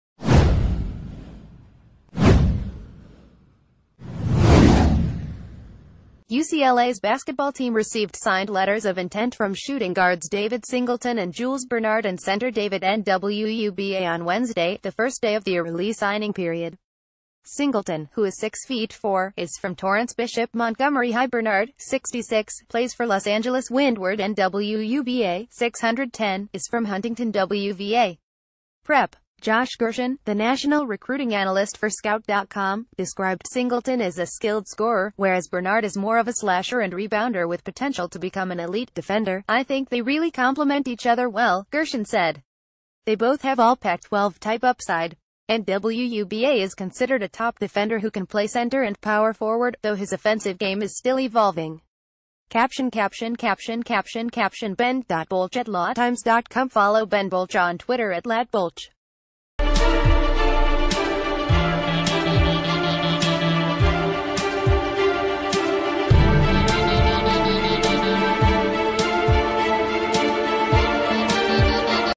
Breaking News | Ucla basketball signs three on first day of early signing period
Ucla basketball signs three on first day of early signing period

UCLA’s basketball team received signed letters of intent from shooting guards David Singleton and Ju...